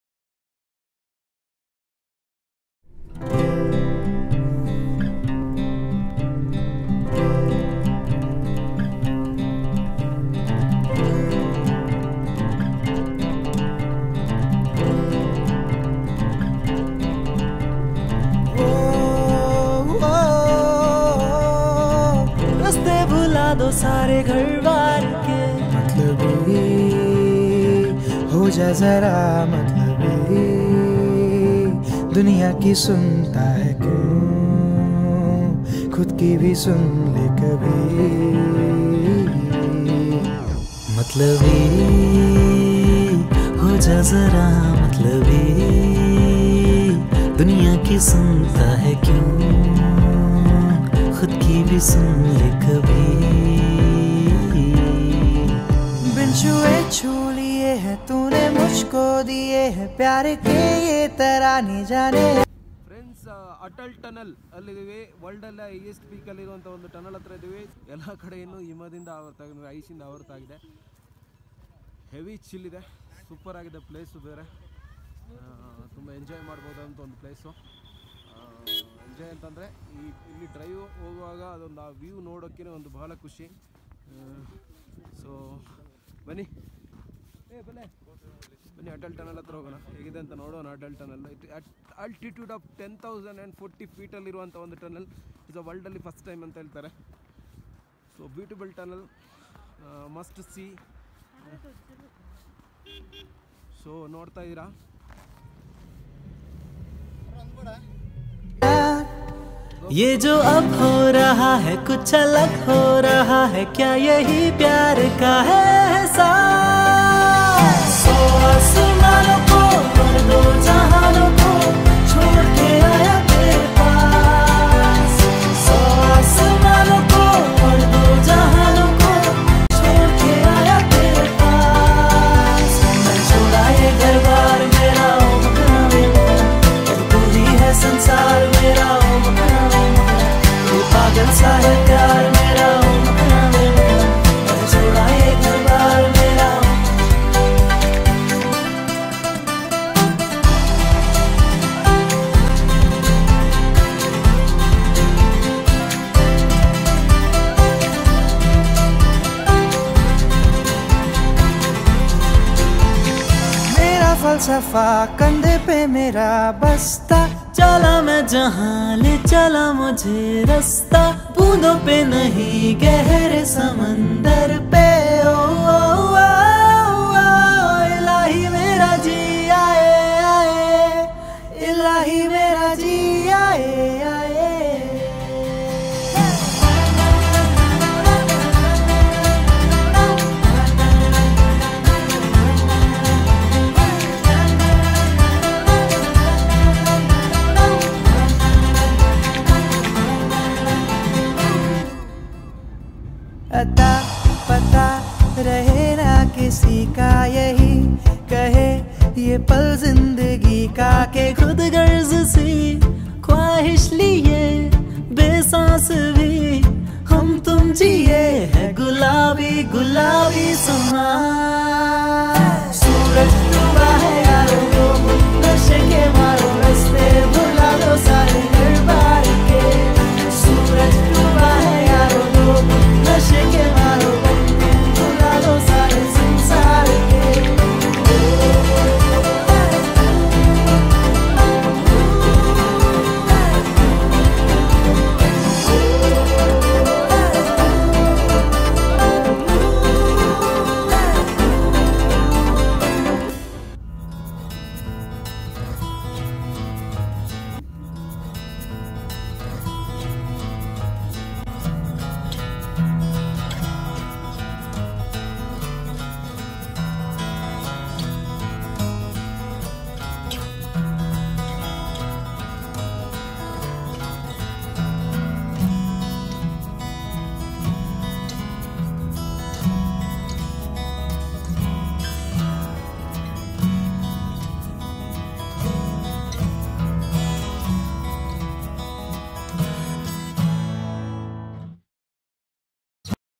Atal Tunnel - Himalaya Worlds highest peak tunnel. Road trip The best view of himalaya.
Atal Tunnel (also known as Rohtang Tunnel)  is a highway tunnel built under the Rohtang Pass in the eastern Pir Panjal range of the Himalayas on the Leh-Manali Highway in Himachal Pradesh, India. At a length of 9.02 km, it is the longest tunnel above 10,000 feet (3,048 m) in the world and is named after former Prime Minister of India, Atal Bihari Vajpayee..

Travel Travel love Vlog Kannada tulu travel mashup Mr. & Mrs. Shetty
Follow Insta : Mr.&Mrs.Shetty